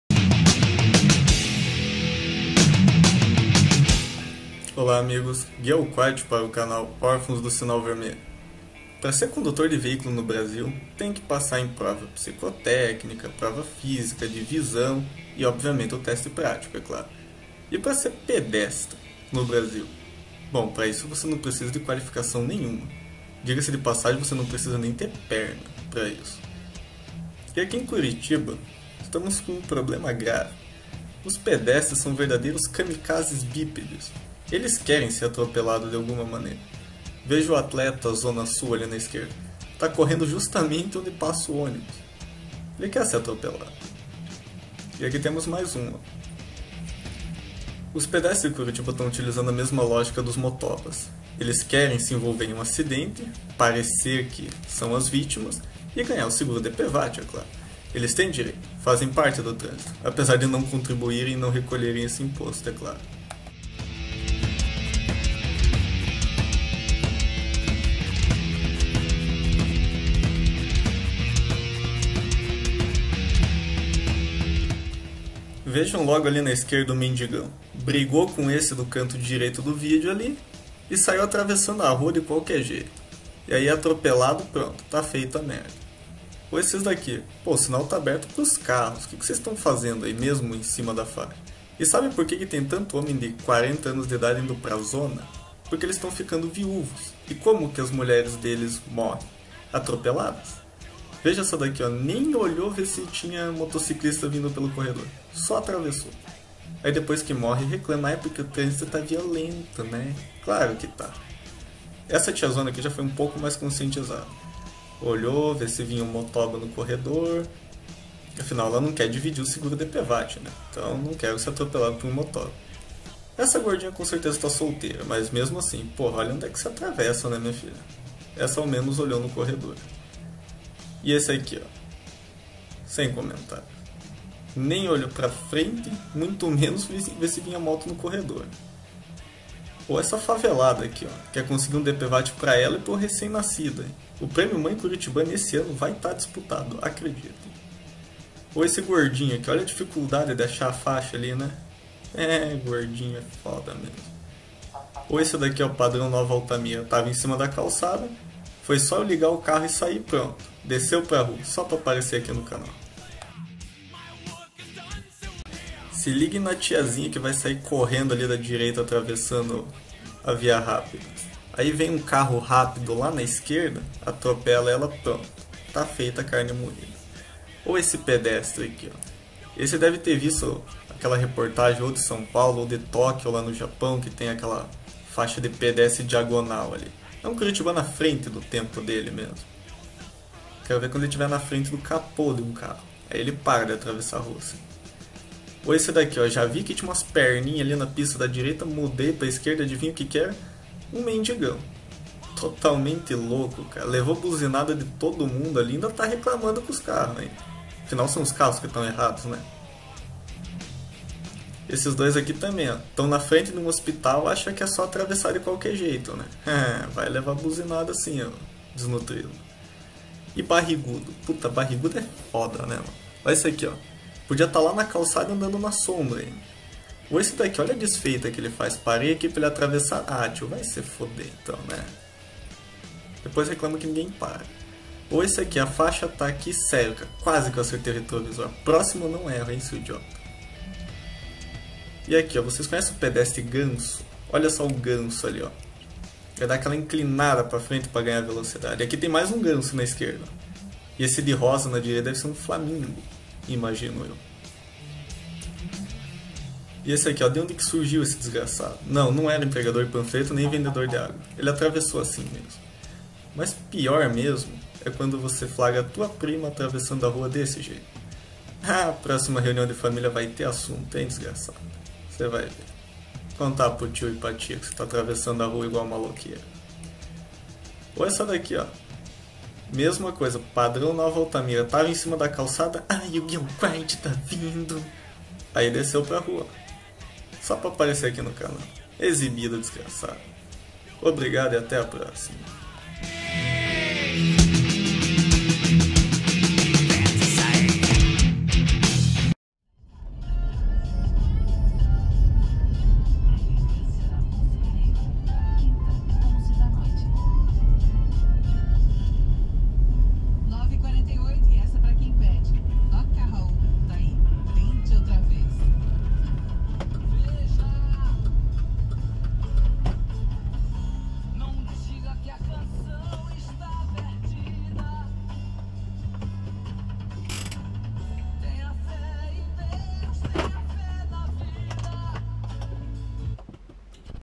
Kamikazes Bípedes by Gui Al Kuwait
Pedestres em Curitiba estão querendo receber indenização do seguro DPVAT. Parecem os motociclistas, mas são conhecidos como Kamikazes Bípedes.